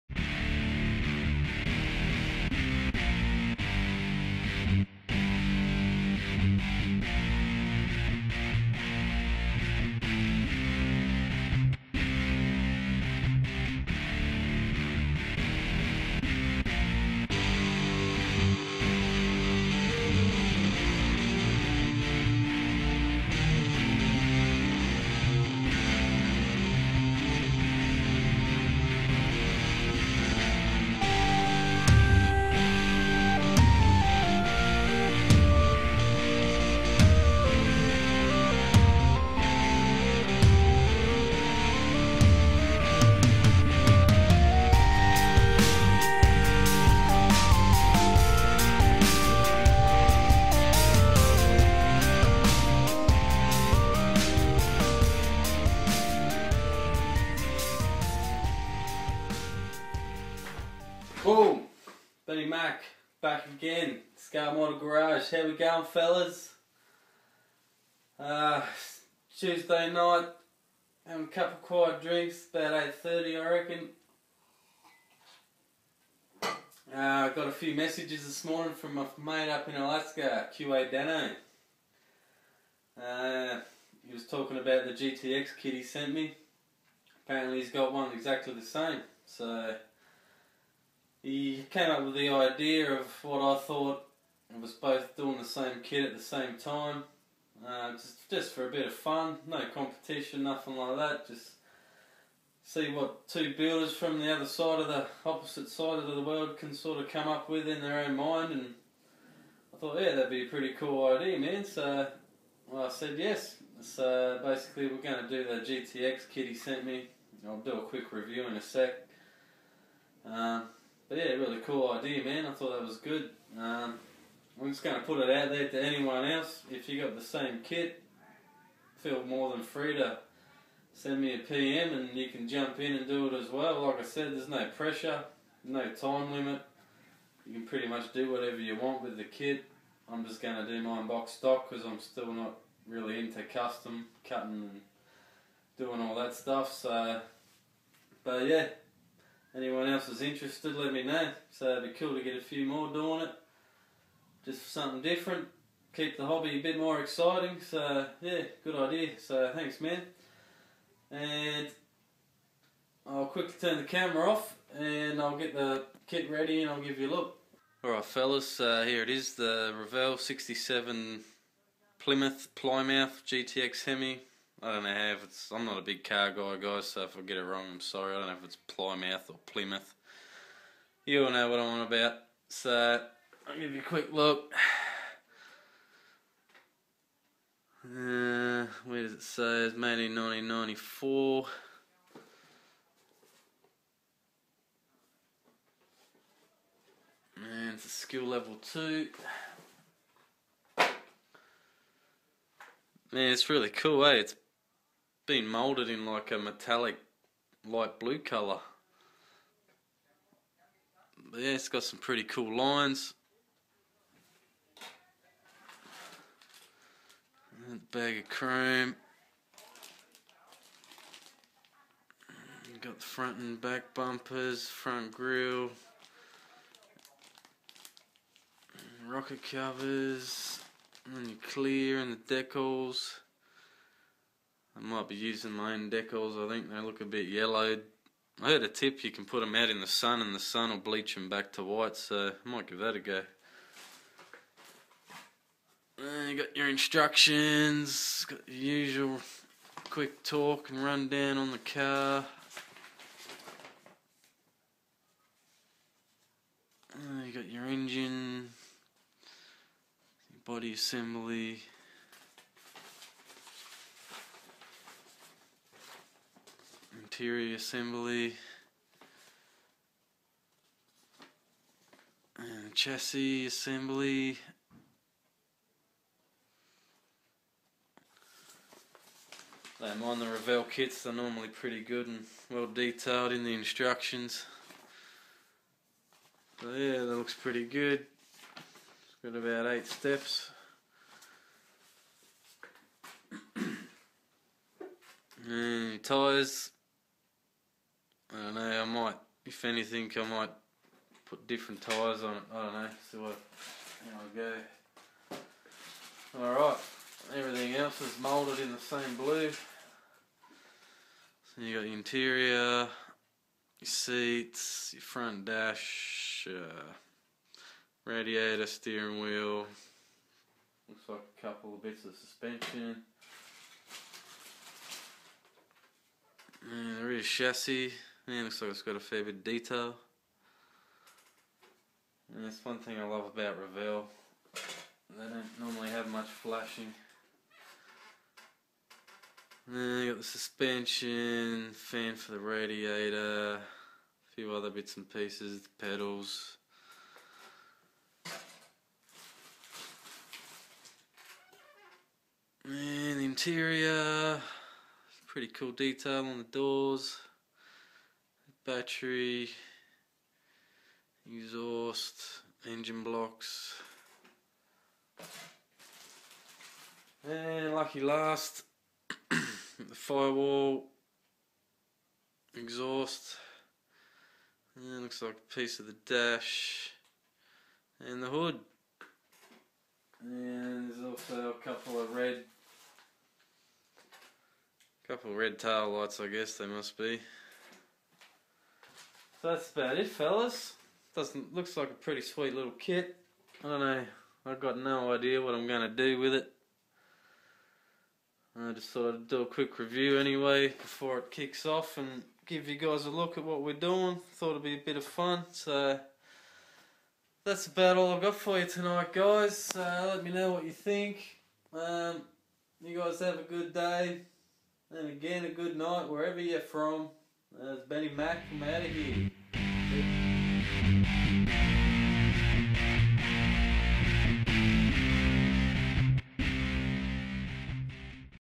Future Build Idea From qadanno, 67 Plymouth GTX Hemi + Quick Review.
So my friend qadanno messaged me asking if i wanted to build the GTX he sent me at the same time he builds his for a bit of fun to see what we could come up with.
If anyone else with this kit wants to join us let me know.
Have a good one all.